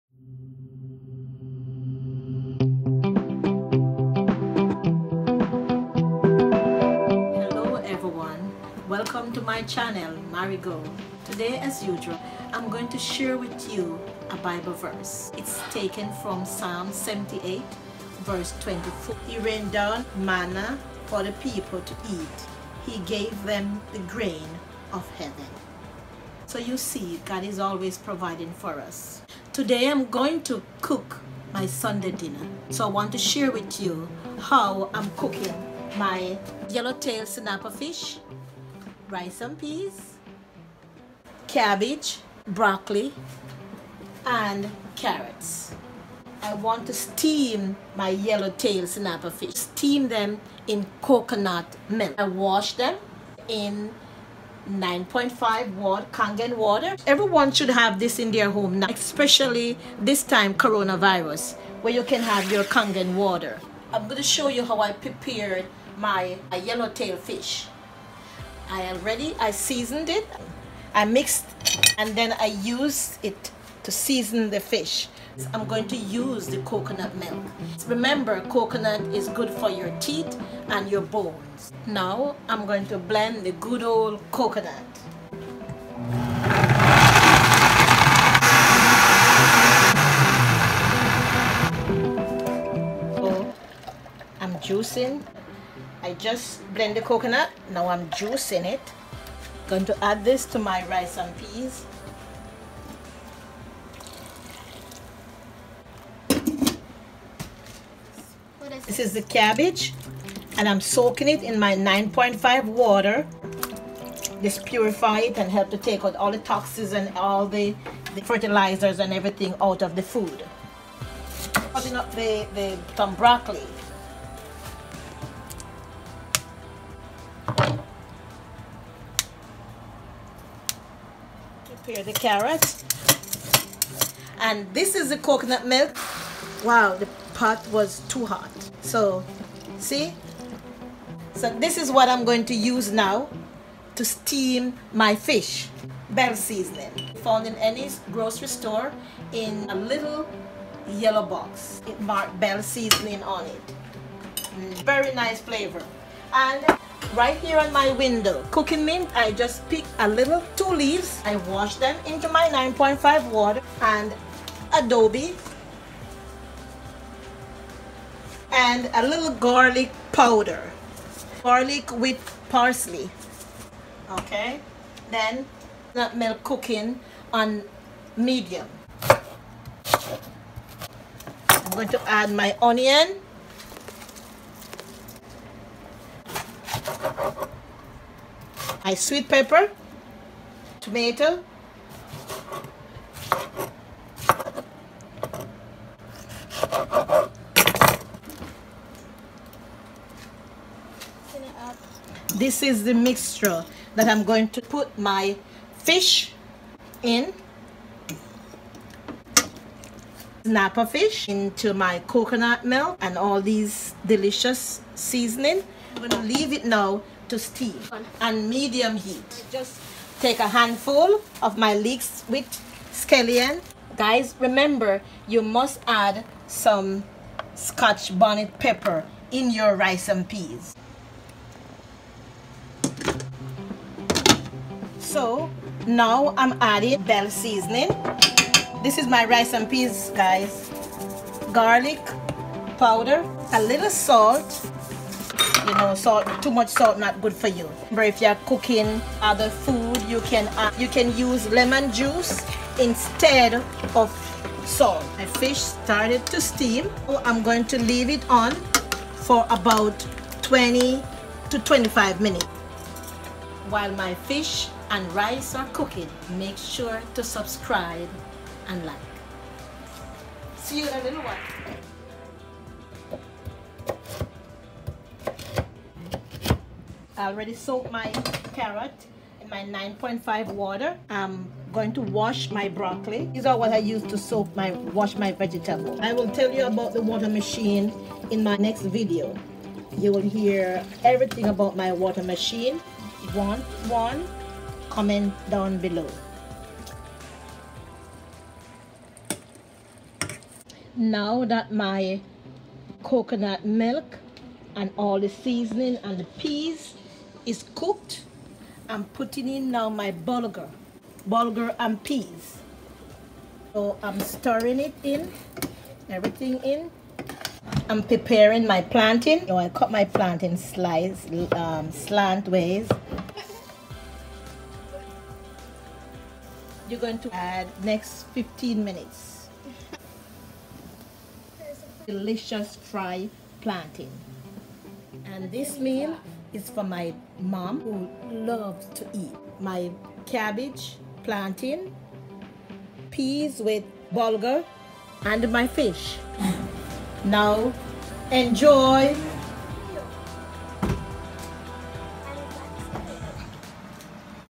COOK STEAMED FISH WITH ME!
I’m sharing with you my delicious Sunday dinner. I cooked steamed fish, veggies, bulgar and peas!